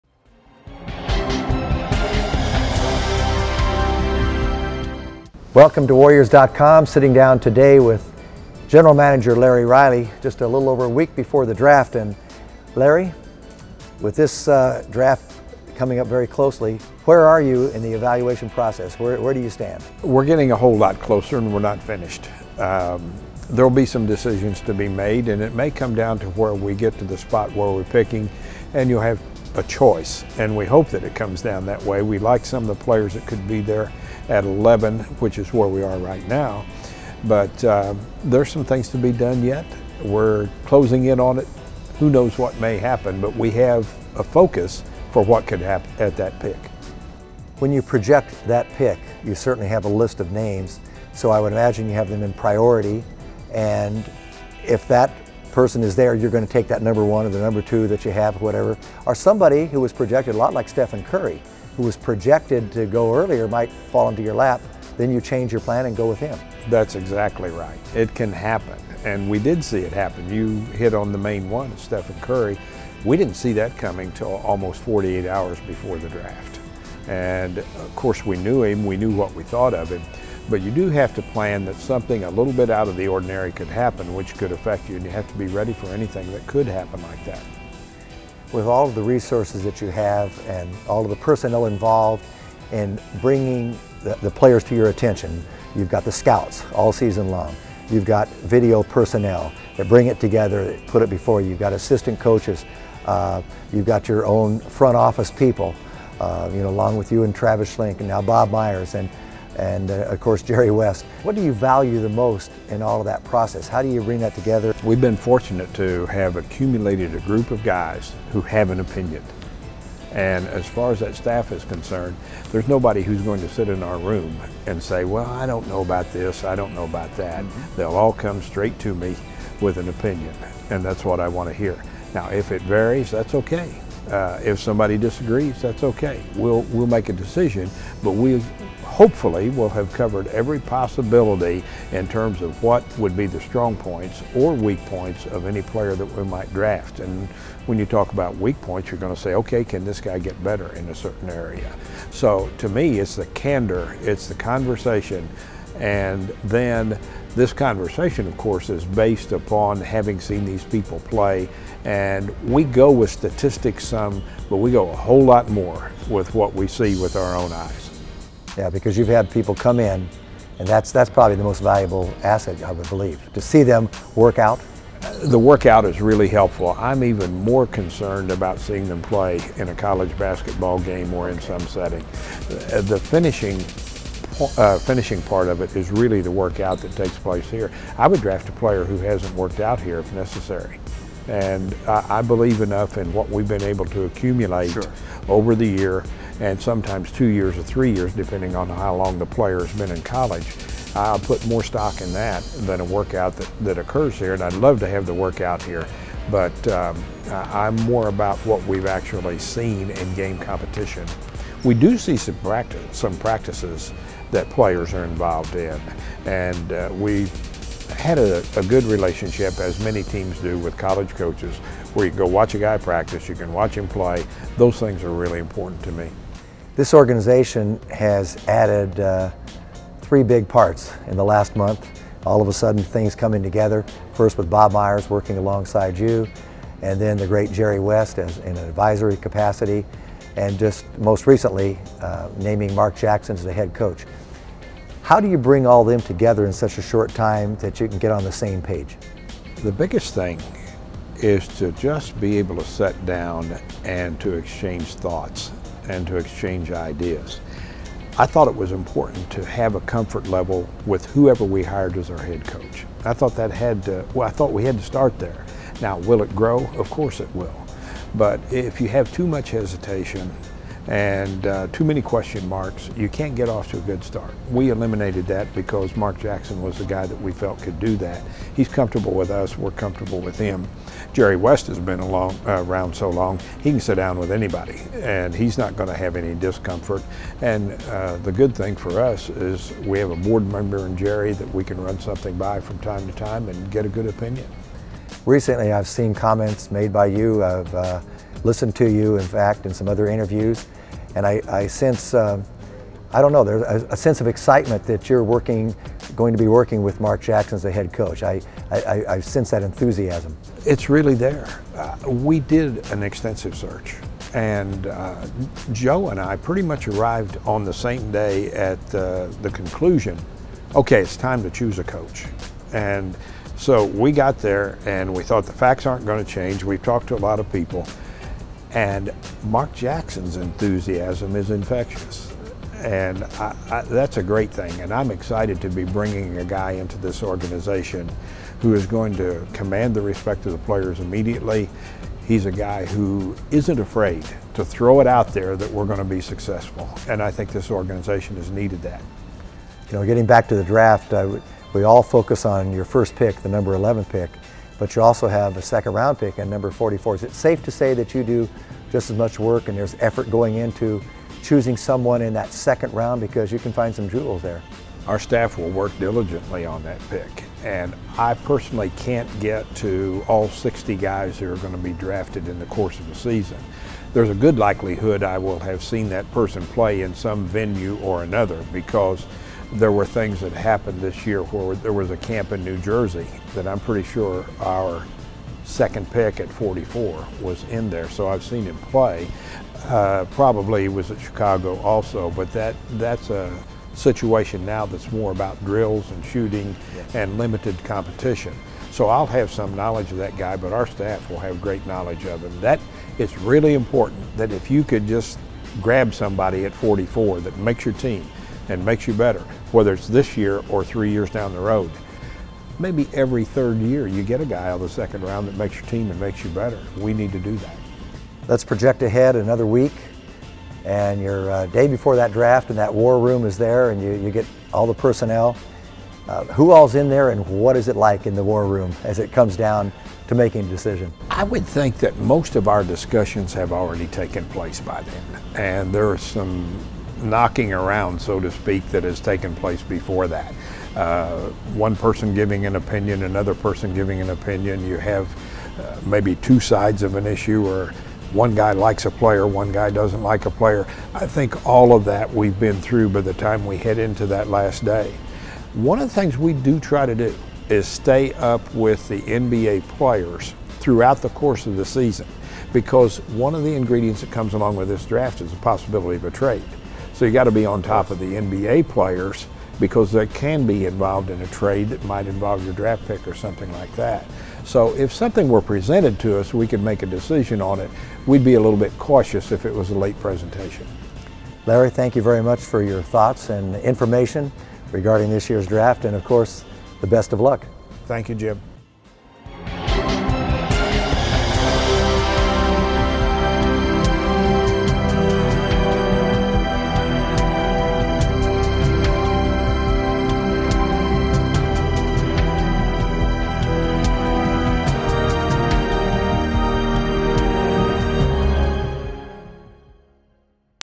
Larry Riley Pre-Draft Interview
Warriors Executive Vice President of Basketball Operations and General Manager Larry Riley speaks candidly with Jim Barnett about the team's ongoing preparation for the 2011 NBA Draft.